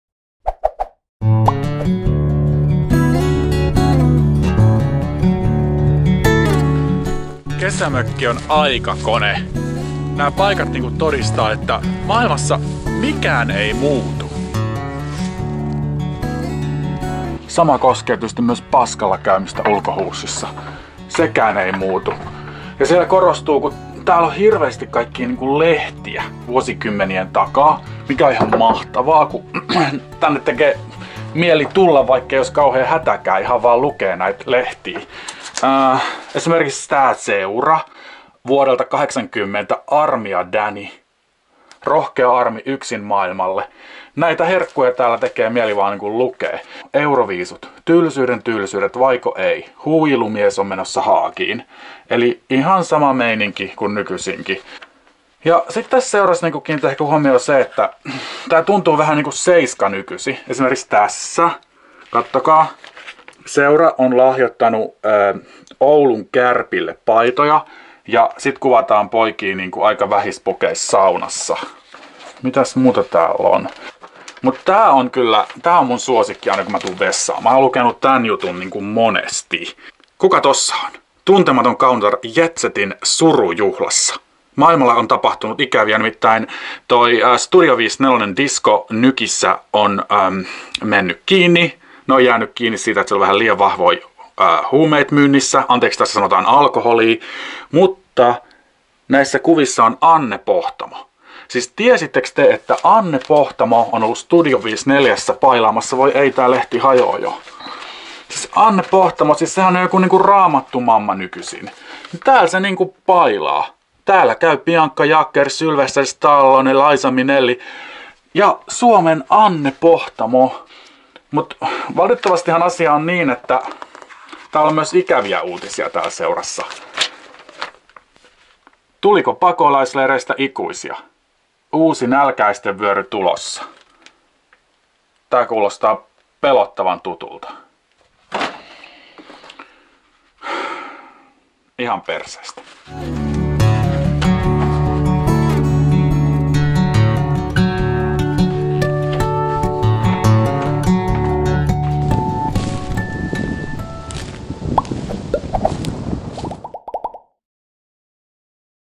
Kakalla. Anne Pohtamo bilettää. Need I say more :)
Kävin ulkohuussissa viime elokuussa ja videoin kokemuksen. Luin ääneen vanhaa Seura-lehteä (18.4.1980). Olisipa aina kesä!